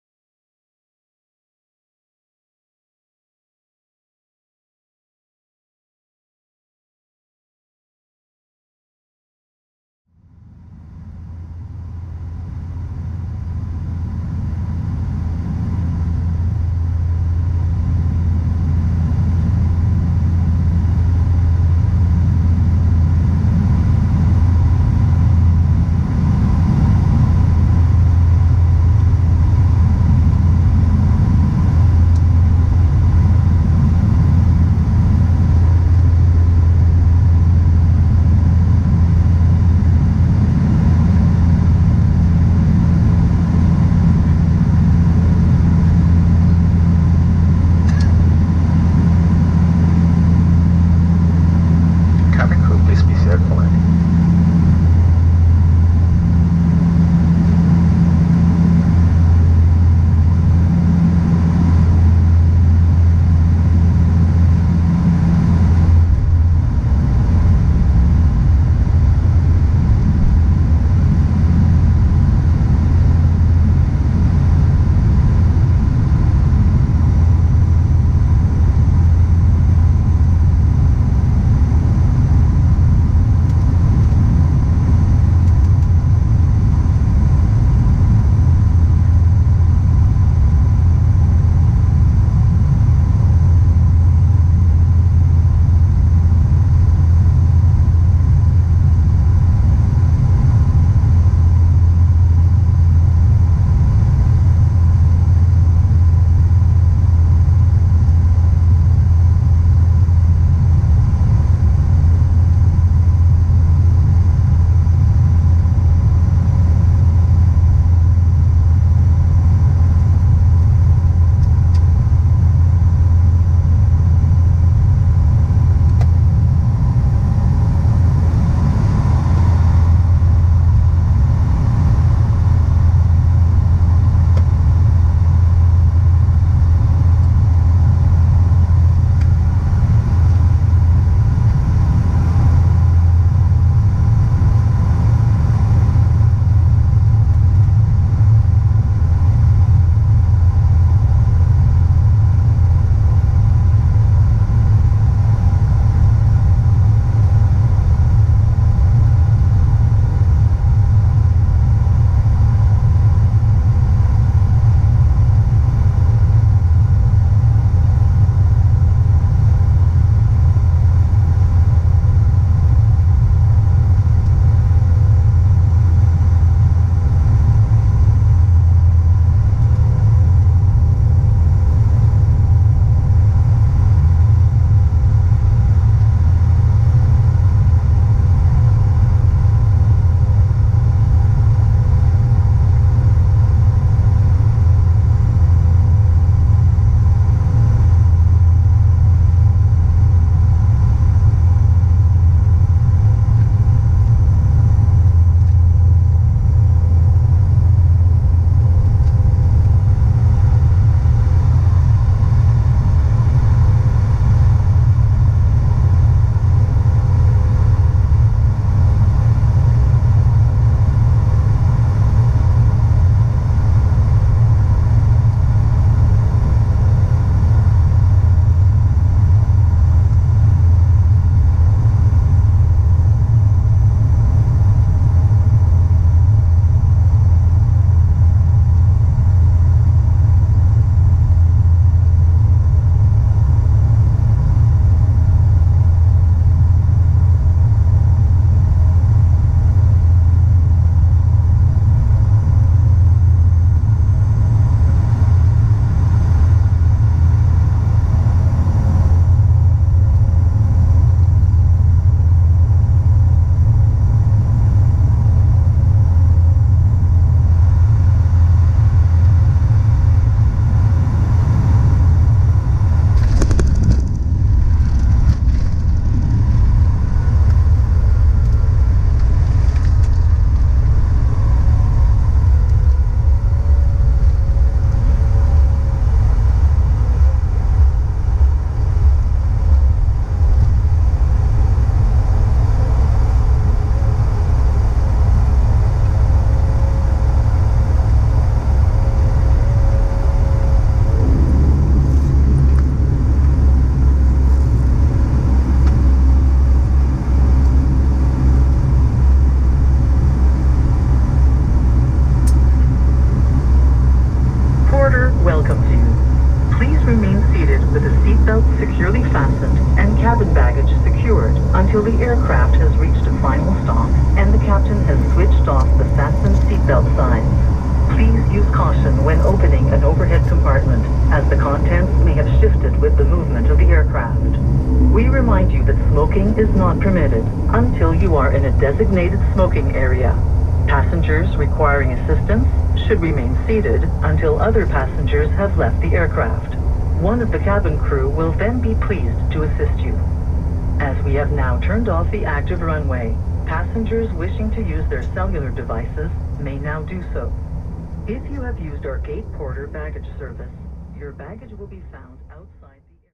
Porter Q400 Landing Toronto Billy Bishop International airport YTZ
Airline: Porter
Aircraft: Q400
Registration: C-GLQB
Date: 08/07/2022
Airport: Toronto Billy Bishop City International airport, Canada
Runway: 26